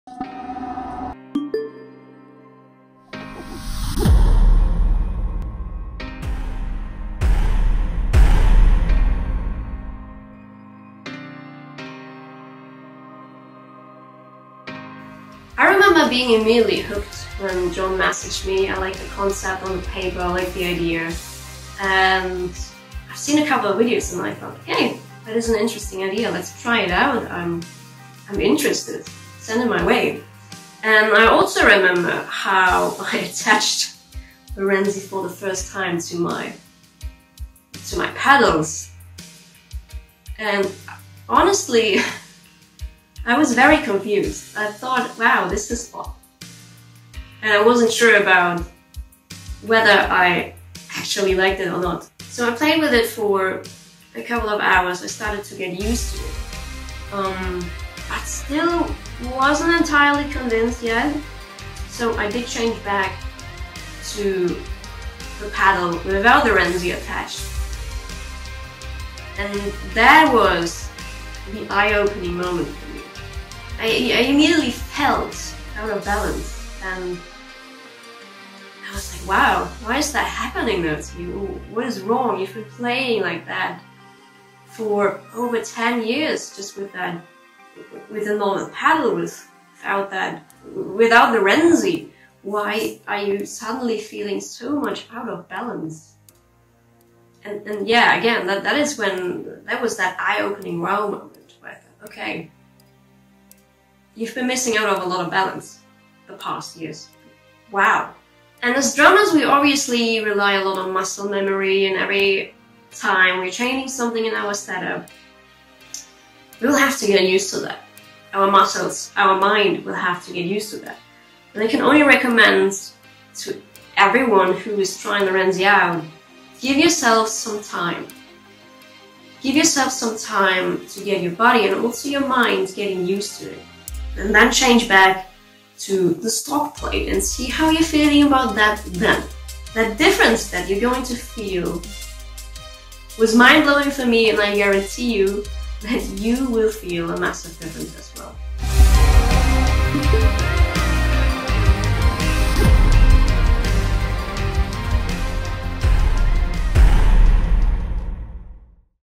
Artist Spotlight: Dana Dohmen - First Impression of The Renzi® – Advanced Heel Pedestal System
Dana Dohmen talks about her first impression of the Renzi Stomp Louder Advanced Heel Pedestal System. The first in its kind, she says "...Wow, I've been missing out on my balance!"